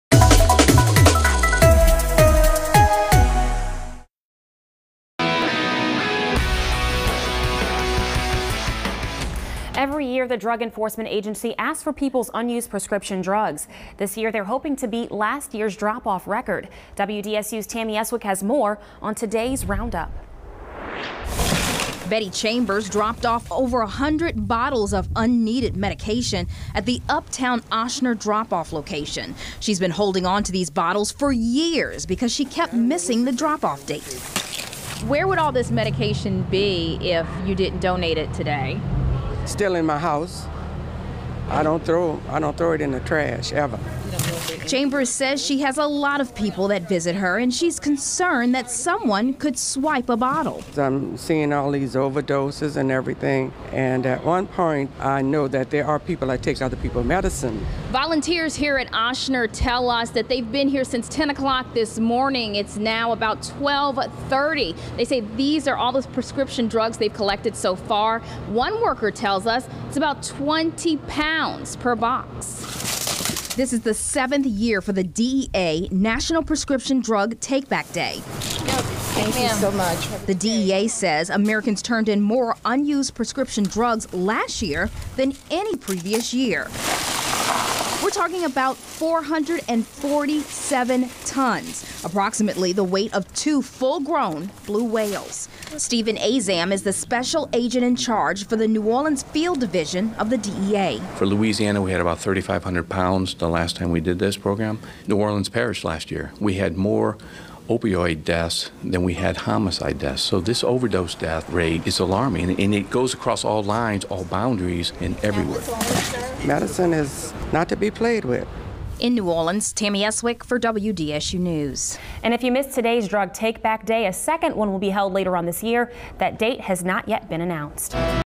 Volunteers collect unwanted, unused prescription medications on Drug Take Back Day
DEA Hosts Prescription Drug Take-Back Day